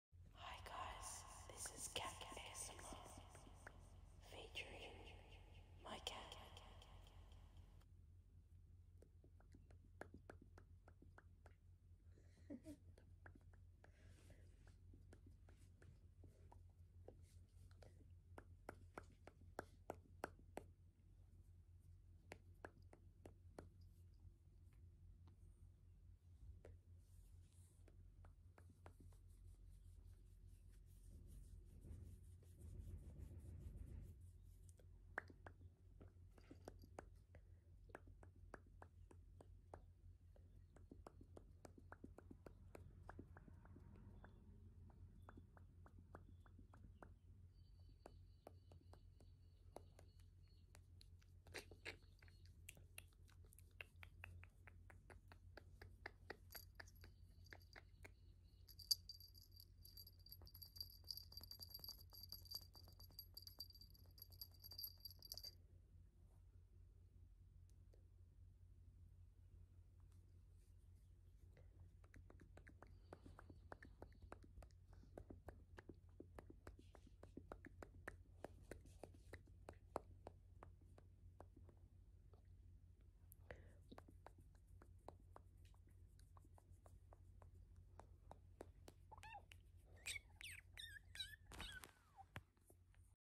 CAT ASMR - SUPER TINGLY 1 minute asmr blast video enjoy :)
Shown in this video is me doing ASMR on my cat￼ My cat is very sleepy because of my ASMR YOU In turn will be very sleepy because of my ASMR thank you and enjoy :-) yasss￼